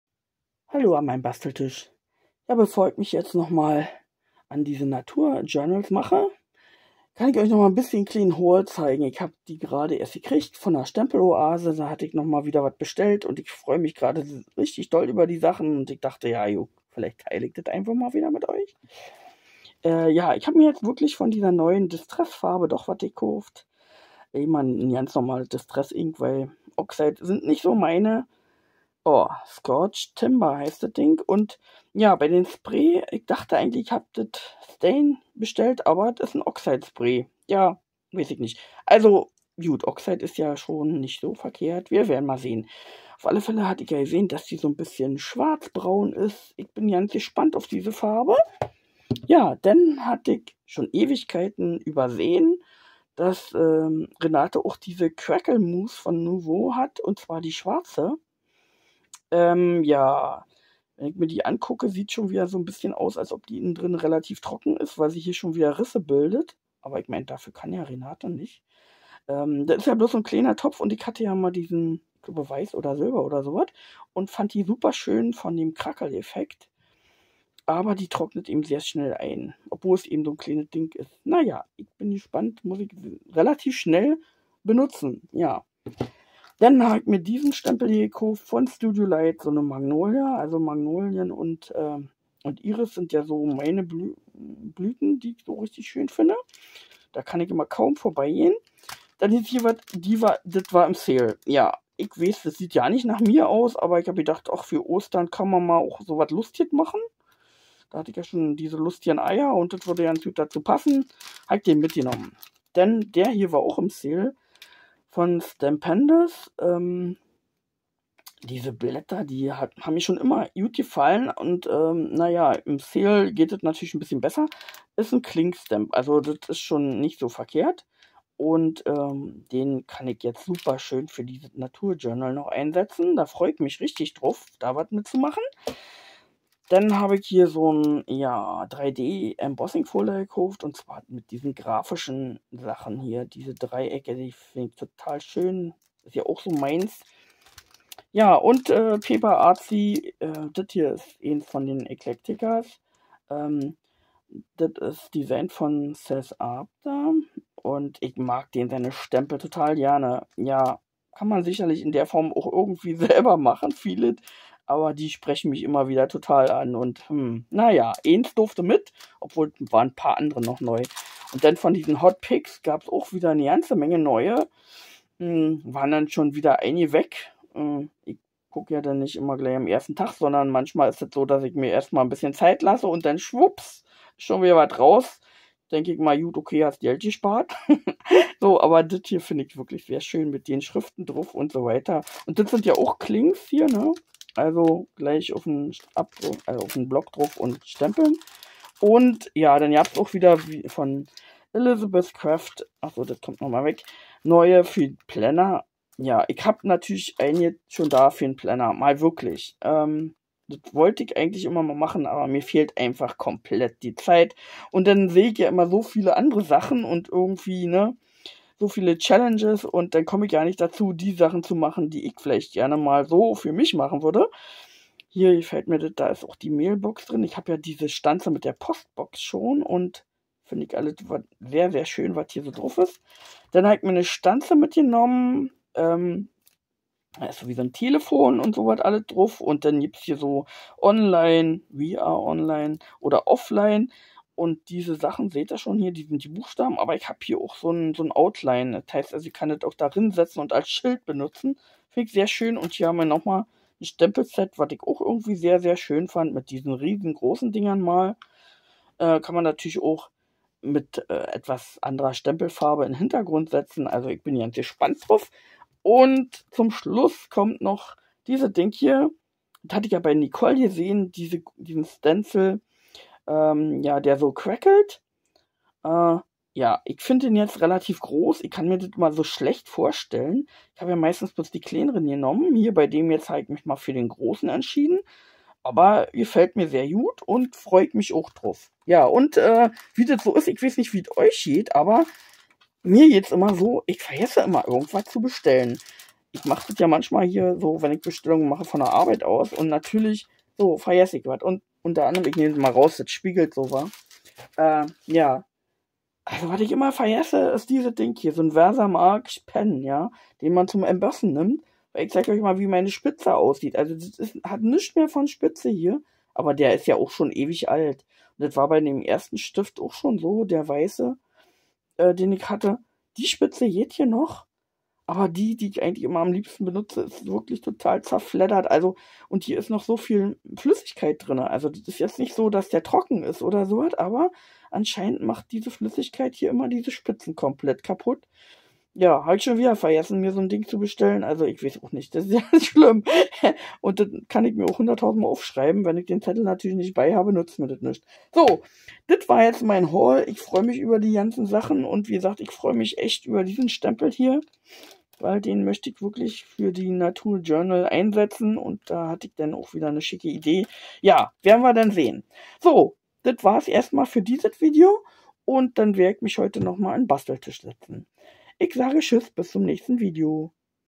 kleiner Haul Stempeloase
Enthält unbezahlte Werbung.
Ein bischen Nachschub an Stempeln u. a. für die Naturjounrals von der Stempeloase. LG Ina